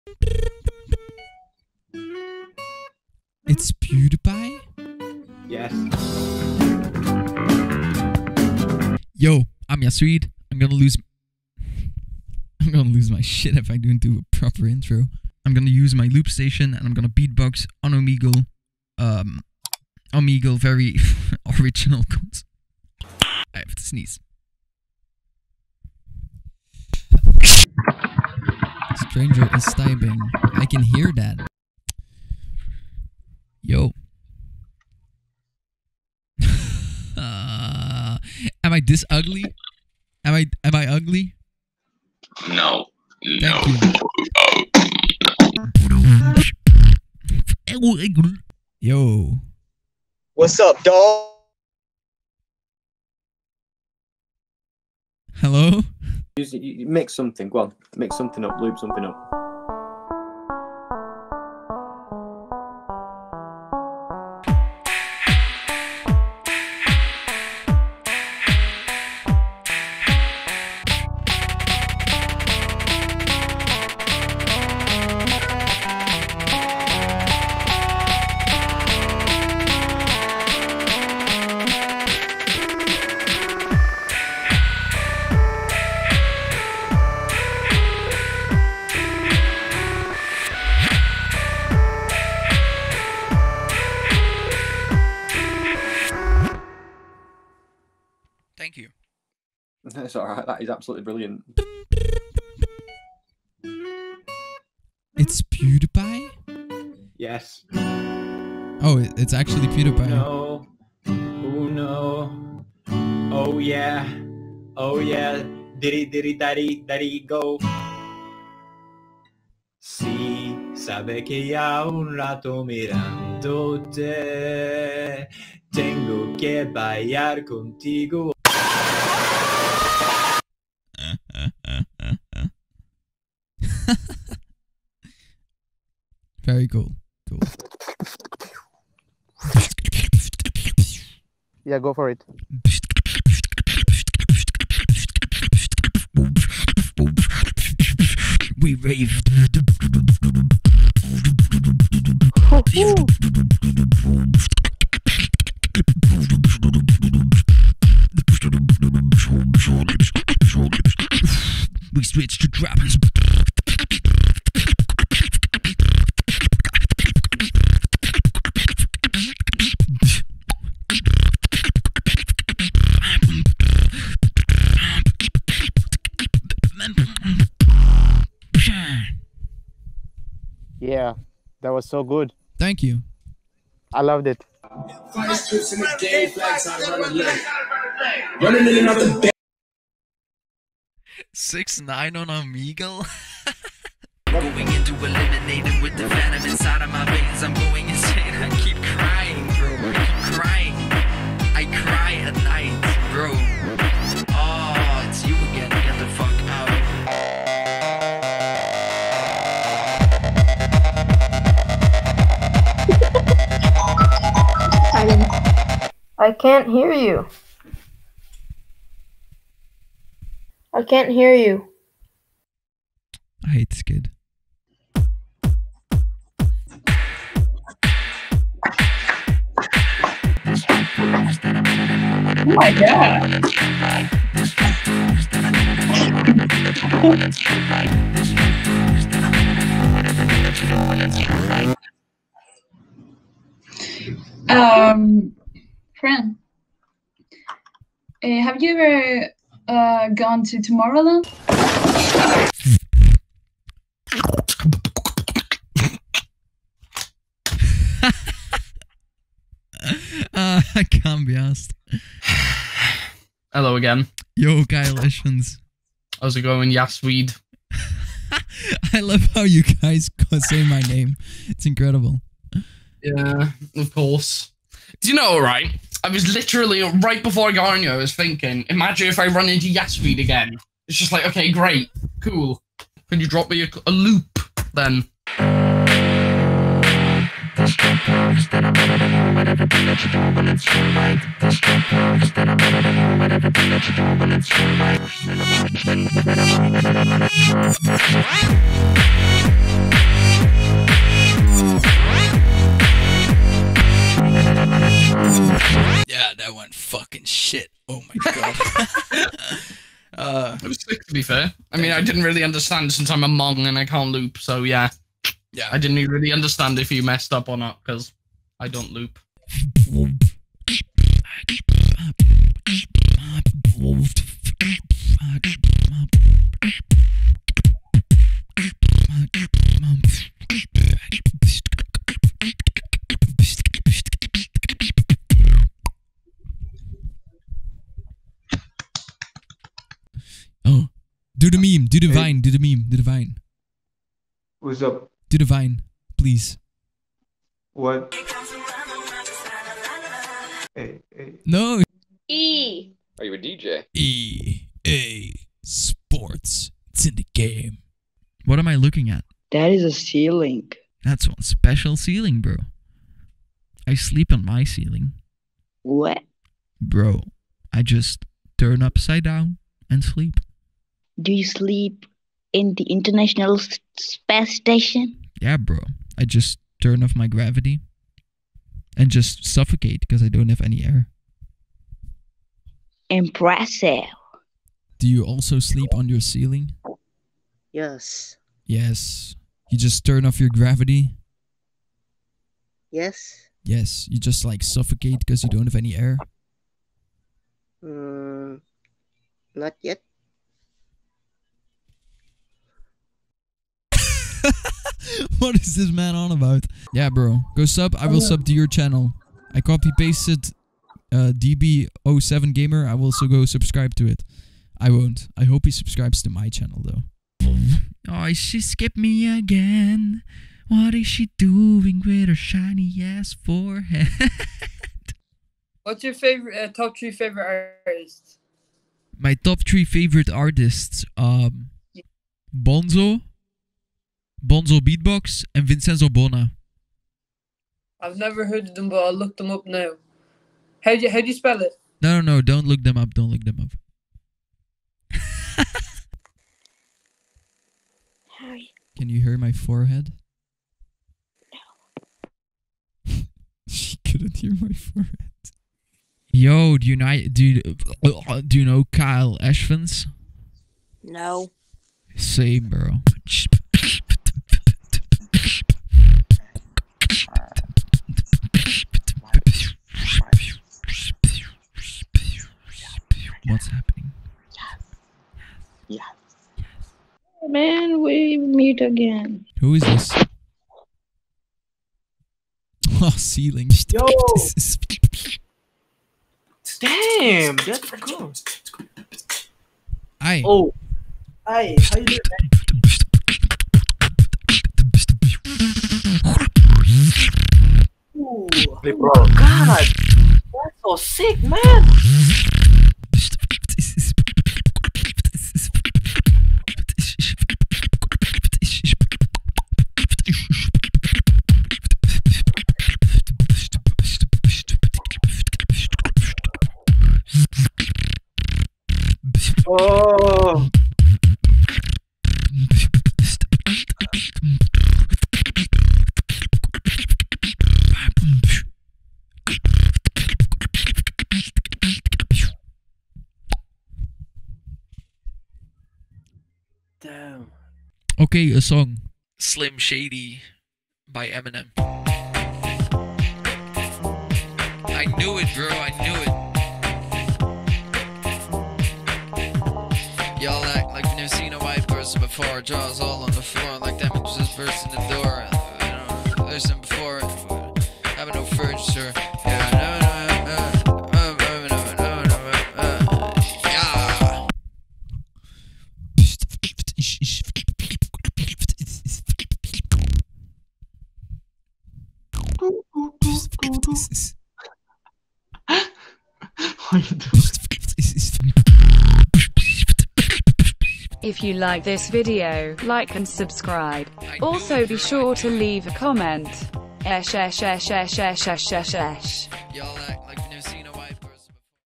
I MET PEWDIEPIE ON OMEGLE?! | BEATBOXING ON OMEGLE #1
I can't believe I found pewdiepie on omegle :O AND kyleEshvans like bruh
ESH ESH ESH ESH ESH ESHESHESH ESH